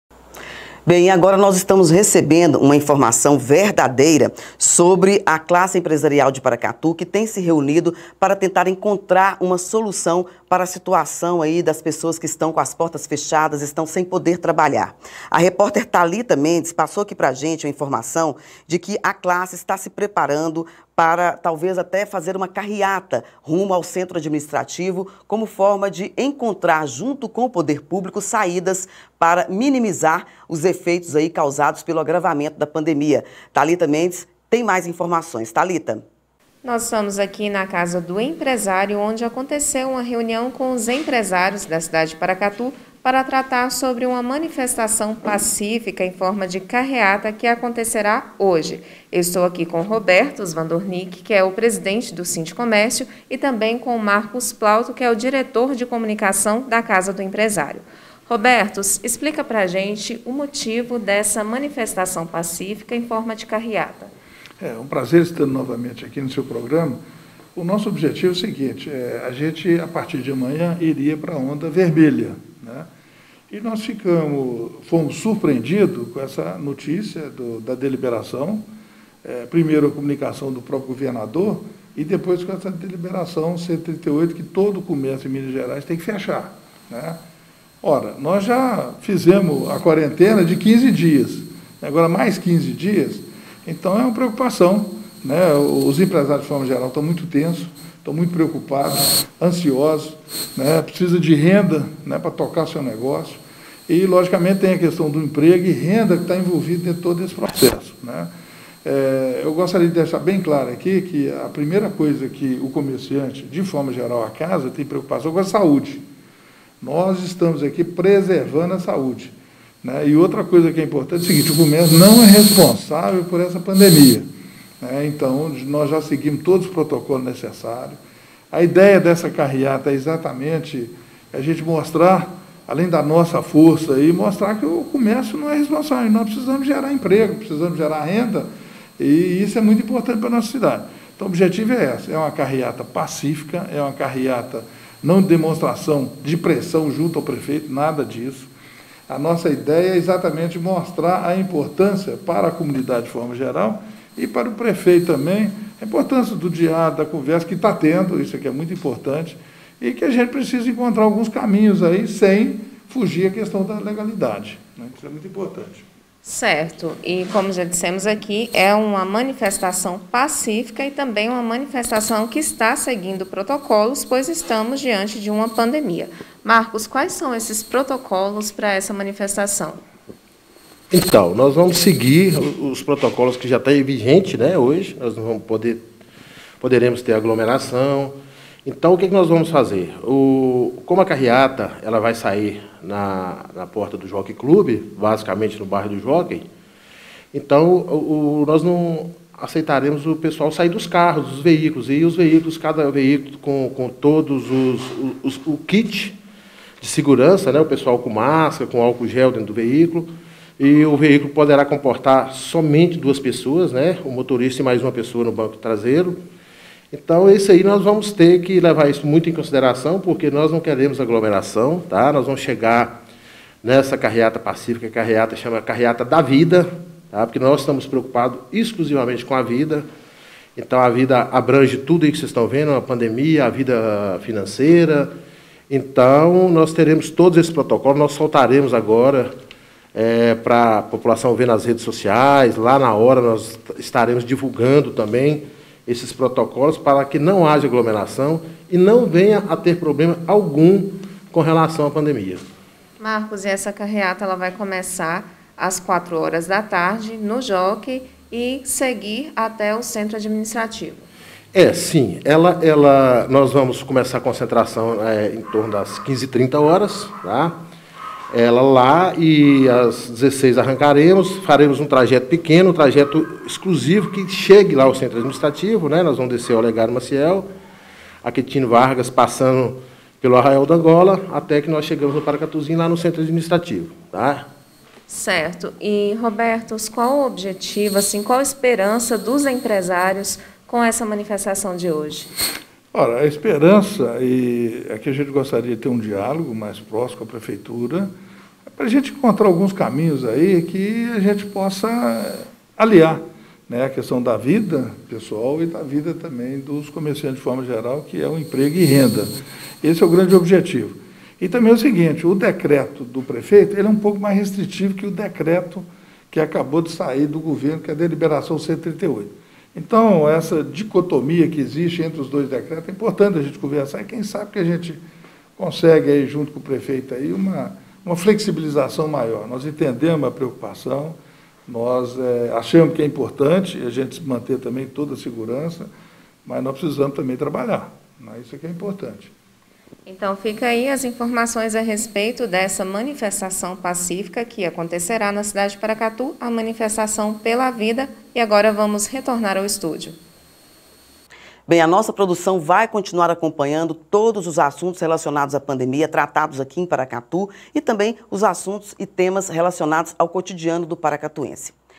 Classe empresarial de Paracatu tem se reunido para buscar alternativas
Nossa produção vai continuar acompanhando todos os assuntos relativos à pandemia em Paracatu.